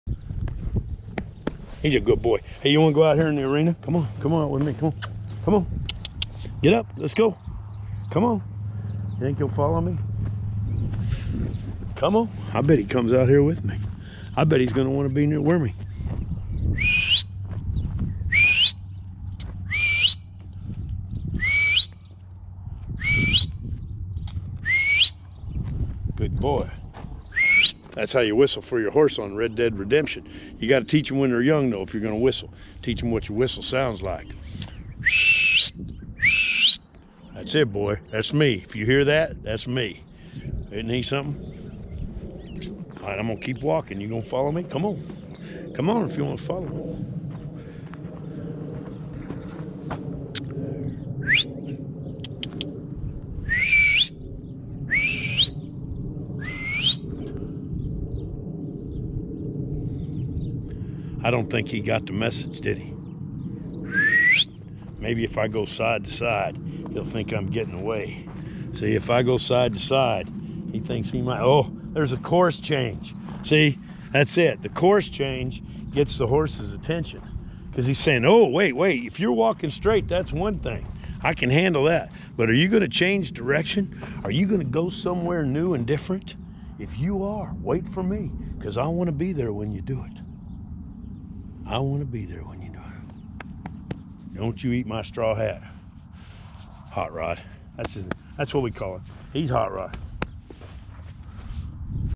hotrodfollow in 4k with HERO 7
yearling follows me about, with lots of snuggles to boot!